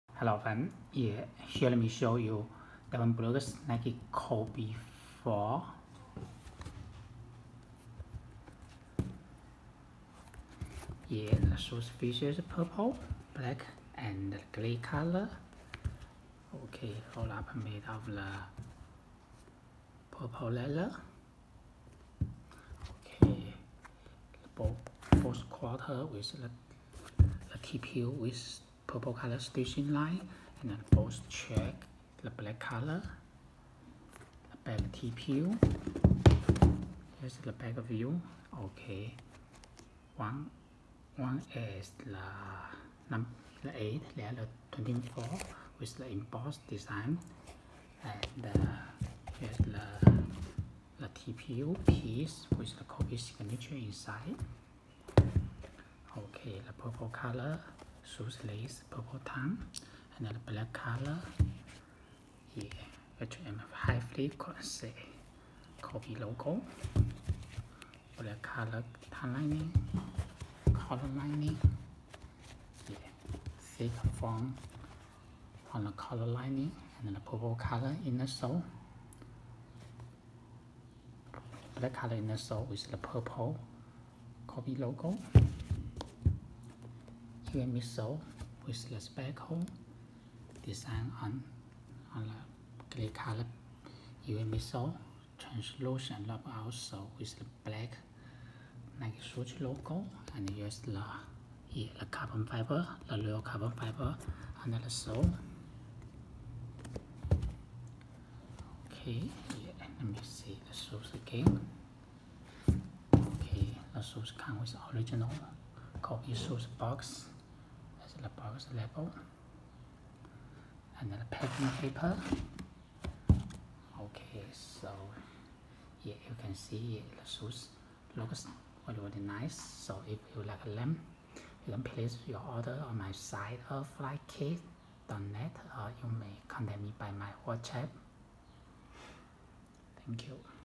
Where To Find Devin Booker's Nike Kobe 4
Devin Booker's Nike Kobe 4

Hey friend today give you the detailed review on Devin Booker's Nike Kobe 4,if you like them,pls click the below link to order on my

A sneaker that Devin Booker wore across two seasons from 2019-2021 is the Nike Kobe 4.
The Nike Kobe 4 originally released for the Black Mamba’s 2008-09 season and was revolutionary for it’s low-cut design. This was a direct request from Kobe Bryant, who saw how soccer players played faster with a lower-cut boot. The Kobe 4s worn by D Book are a “Protro” version, which slimmed the heel shape and increased the range of the traction. The heel Zoom Air and soft phylon foam remained in the cushioning, providing lightweight bounce when landing on the heel. The upper features synthetic leather uppers, with Flywire panels in the midfoot for added lockdown.
D-Book notably wore PEs of the Kobe 4 Protro, although his UNDFTD collab Suns colorway did release .The Phoenix guard also wore his violet PE his first game since Kobe’s passing in January 2020. Booker busted out the “Fade to Black” version back in 2016 as well.